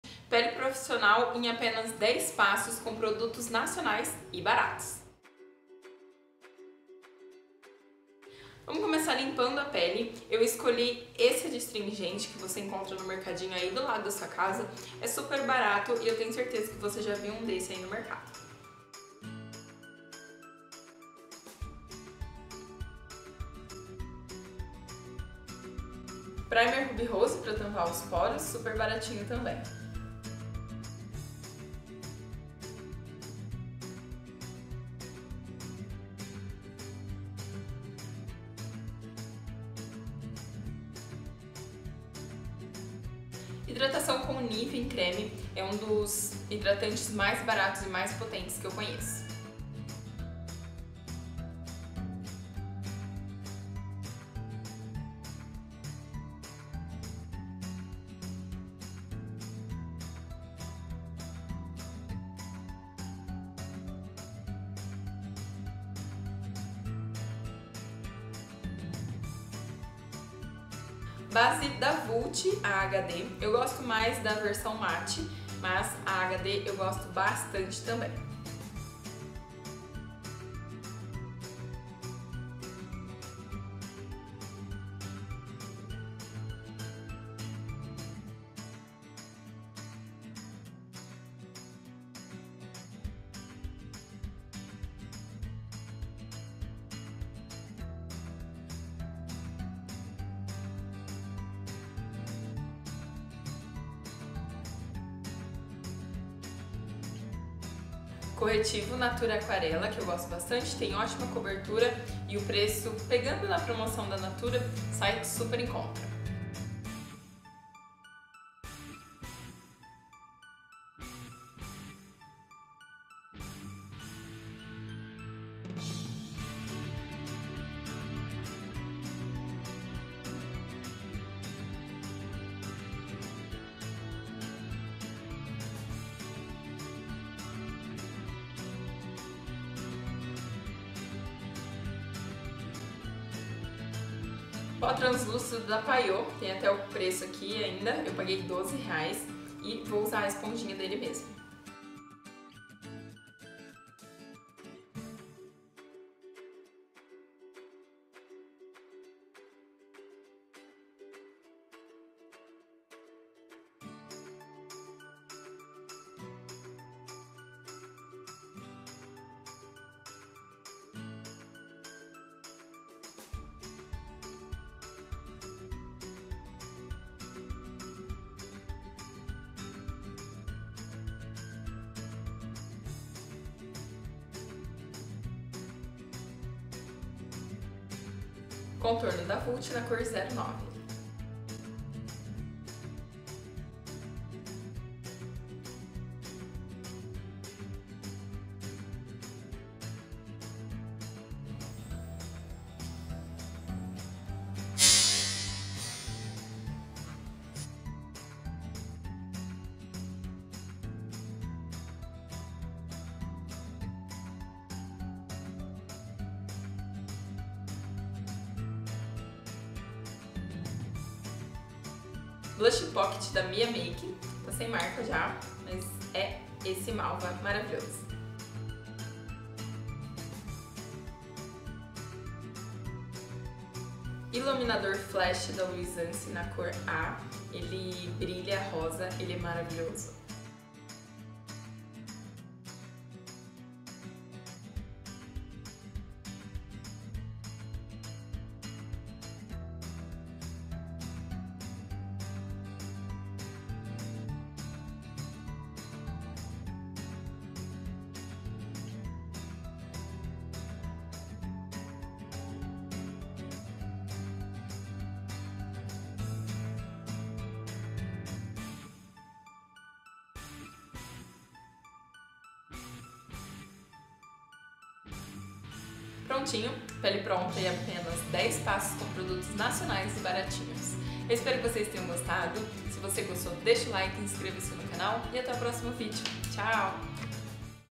10 PASSOS PARA UMA PELE PROFISSIONAL COM PRODUTOS NACIONAIS E BARATOS
PRODUTOS BOM BARATO E NACIONAL? VEM QUE EU TE ENSINO RAPIDINHO a fazer uma pele profissional